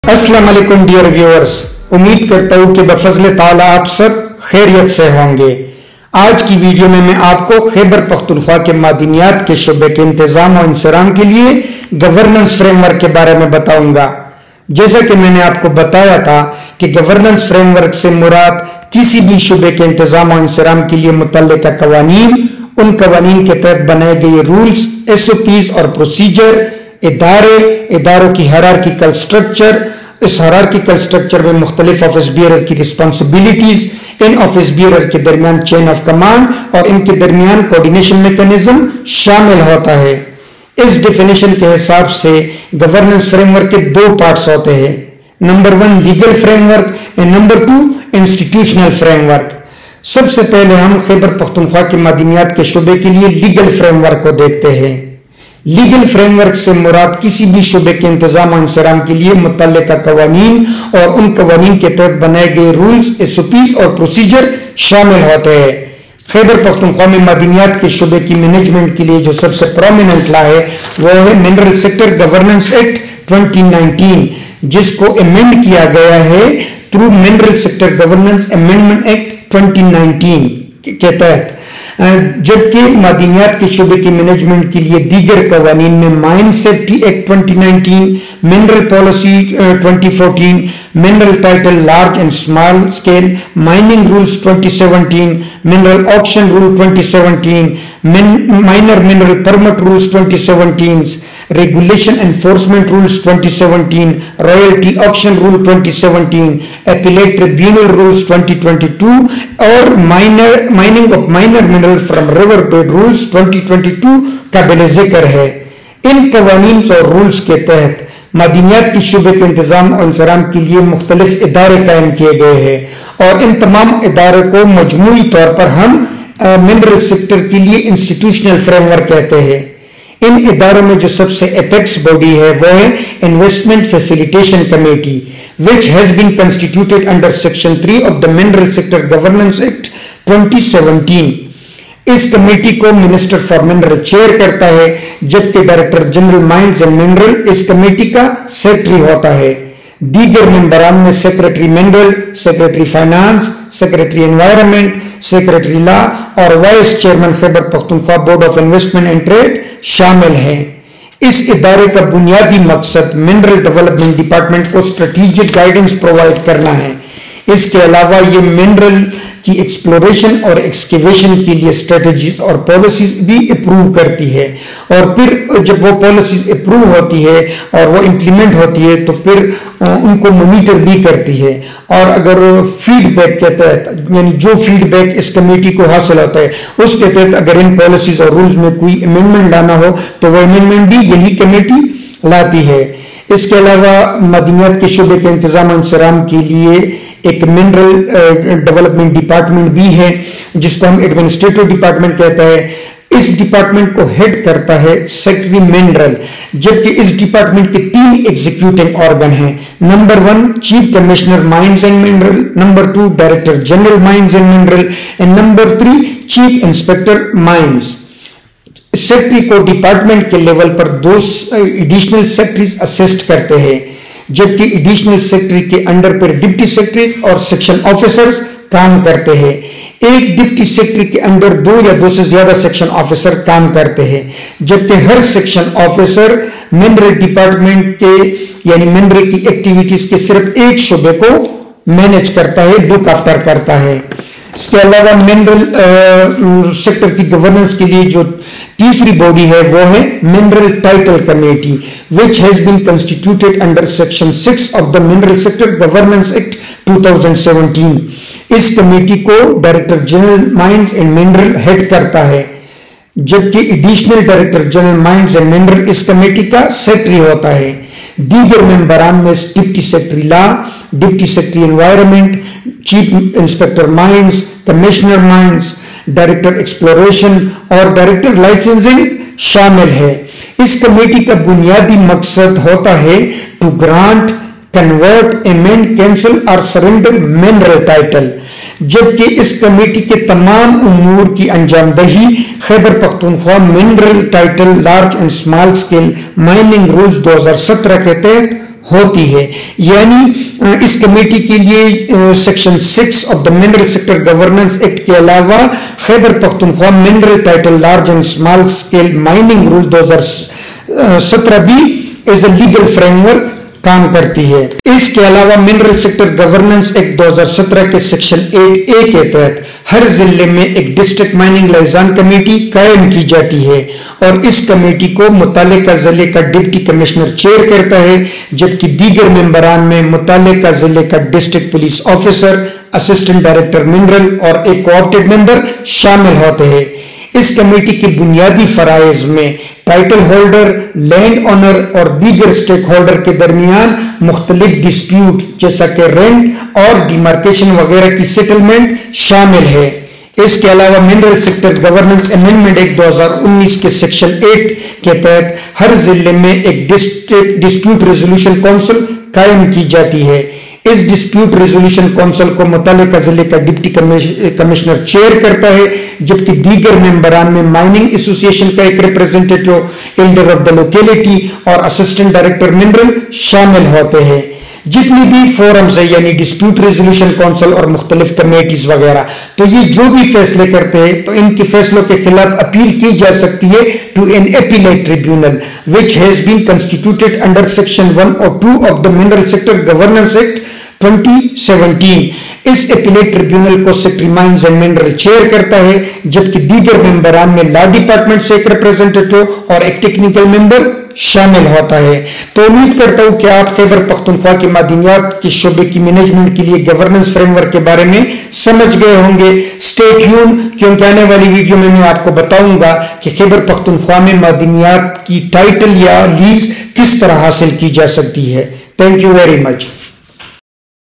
Mineral Sector Governance Laws And Institutions: Explaining KP Mineral Sector Management!
Welcome to Versa Learn Hub! Journey through the intricate legal fabric of Khyber Pakhtunkhwa's mineral sector with Versa Learn Hub's exhaustive examination of its comprehensive legal and institutional frameworks. Unravel the labyrinthine legislation that governs the extraction, management, and oversight of minerals like chromite, marble, limestone, coal, gemstones, copper, iron ore, manganese, chromium, nickel, tungsten, lead, zinc, silver, and gold, ensuring ethical and sustainable practices in the province.
Key Laws and Rules Explored:
Mineral Sector Governance Act (2017): Establishes a comprehensive framework for the governance of mineral resources, ensuring transparency and accountability in the sector. Regulates the exploration, extraction, and management of minerals, and sets out the roles and responsibilities of various stakeholders.
Mines Safety Act (2019): Ensures the safety and health of mine workers, and regulates the safety standards and protocols in the mining industry. Aims to prevent accidents and injuries in mines, and provides for enforcement mechanisms to ensure compliance.
Mineral Policy (2014): Outlines the provincial government's vision and strategy for the development of the mineral sector, aiming to promote sustainable development, increase investment, and enhance the contribution of the sector to the economy.
Mineral Title (Large & Small Scale) Mining Rules 2017
Mineral Auction Rules 2017
Minor Minerals Permit Rules 2017
Regulations Enforcement Rules 2017
Royalty Auction Rules 2017
Appellate Tribunal Rules 2022
Mining of Minor Minerals from River Bed Rules 2022
Prominent Institutions Discussed:
Investment Facilitation Committee
Mineral Development Department
Mineral Title Committee
Disputes Resolution Council
Appellate Tribunal
District Mining Liaison Committee
Join us as we meticulously dissect the legal and institutional intricacies shaping the mineral sector landscape in Khyber Pakhtunkhwa. Gain profound insights into the governance dynamics that underpin sustainable mineral resource management.